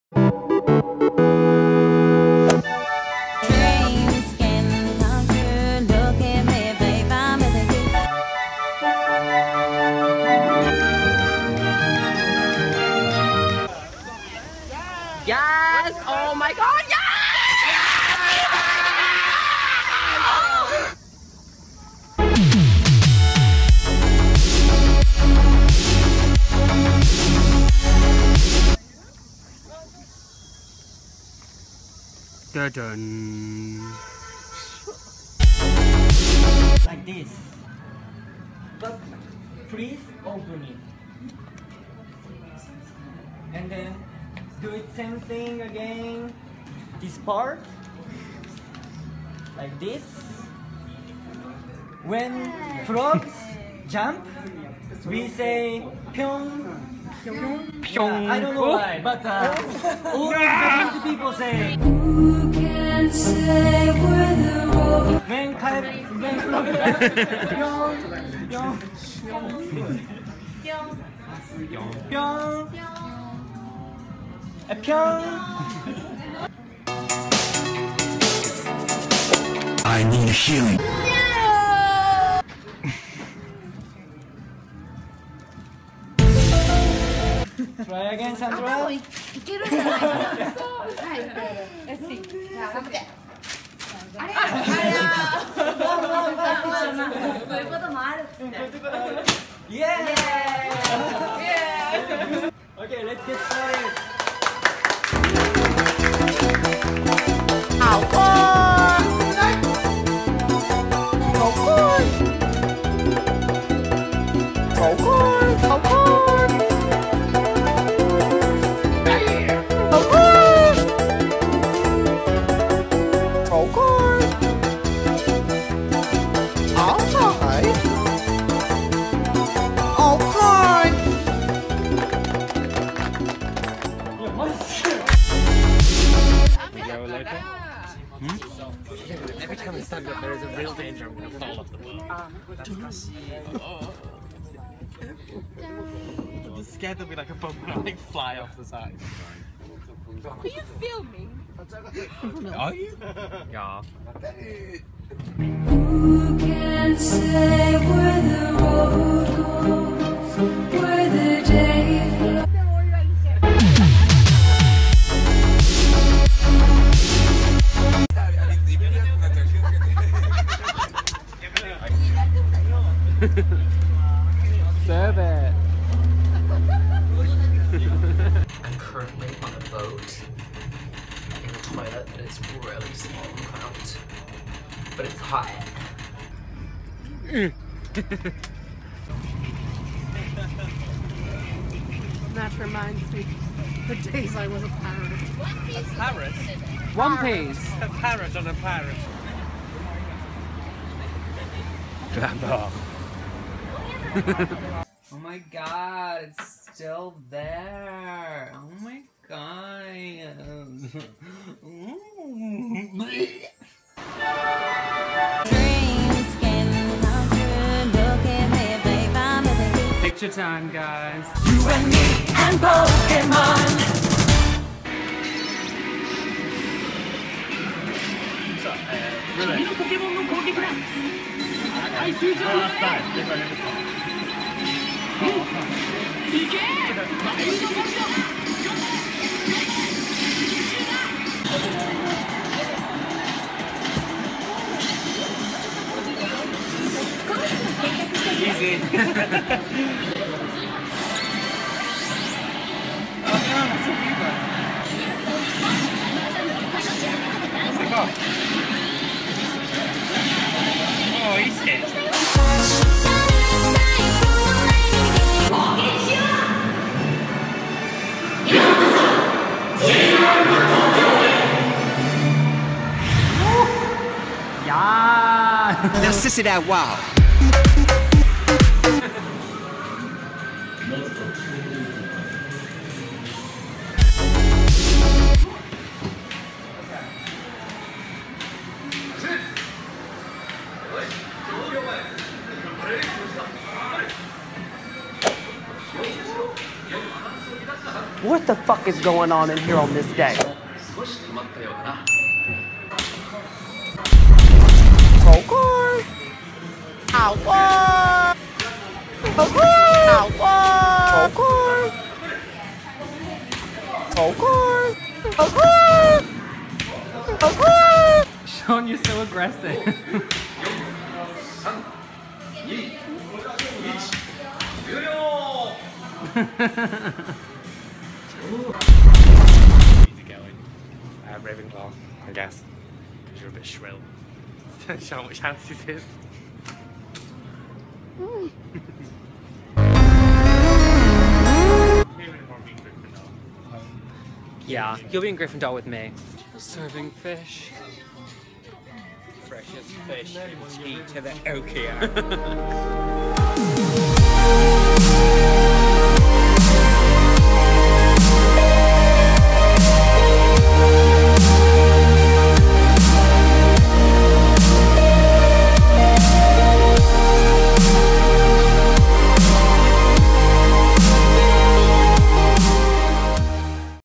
Japan Vlog #3 | Origami Masters and Anime In Real Life
Even more adventures around the wonderful world of Tokyo Japan, witness the wonders of the Ghibli museum as well as some hot daddies along the way all why trying to become a Pokemon master.

Social Media Links:

Snapchat. ►andyrooalert